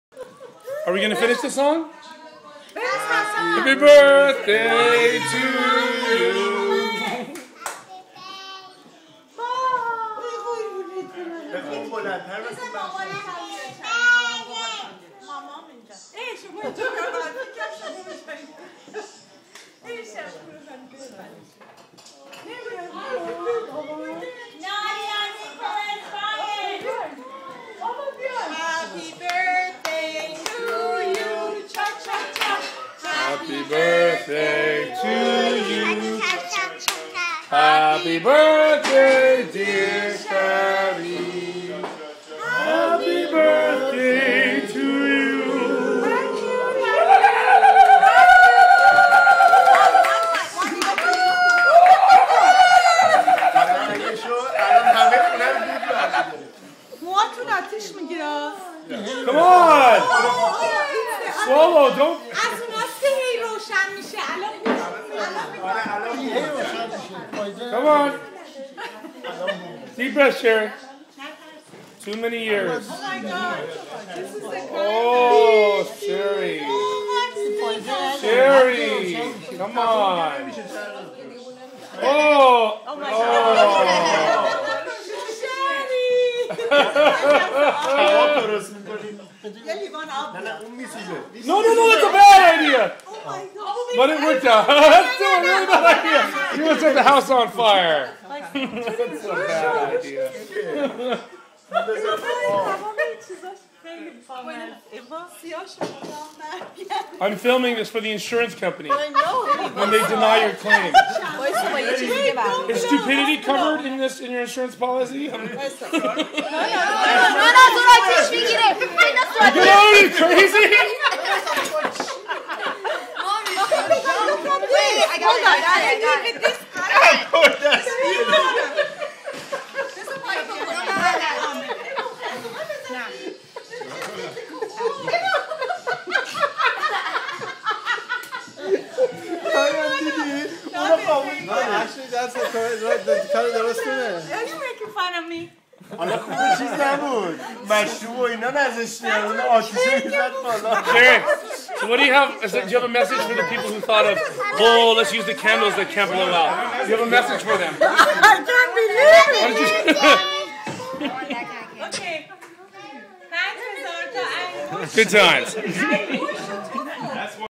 Happy Birthday Shari Gonzalez
Happy Birthday Shari Gonzalez. Shari Gonzalez 60th Birthday Candles wont blow out